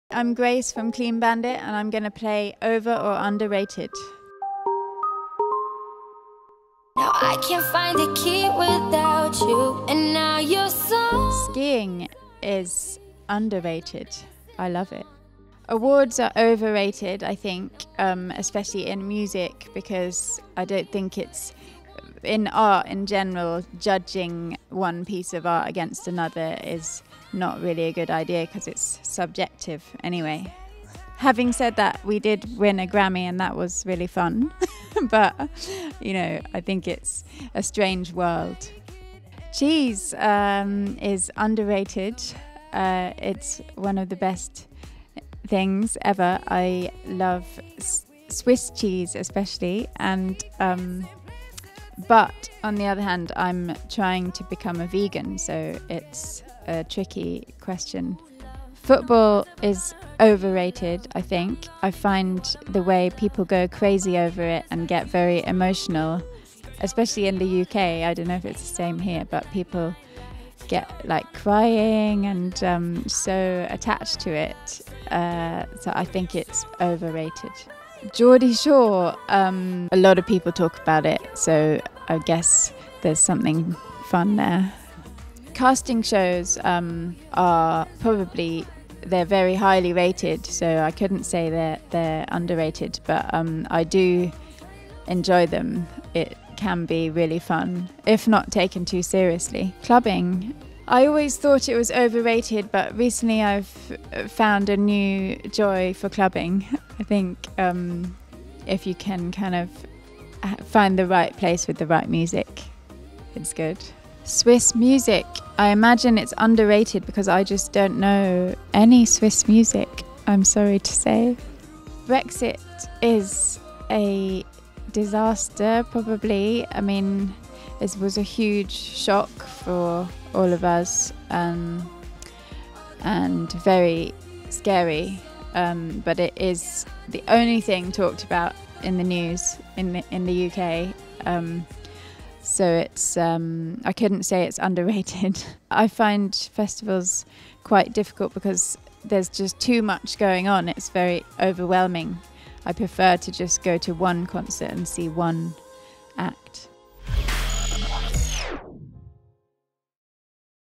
Clean Bandit | über oder unterbewertet | Ticketcorner
Wir haben die Cellistin Grace Chatto von Clean Bandit zu einer Runde «Über- oder unterbewertet?» getroffen.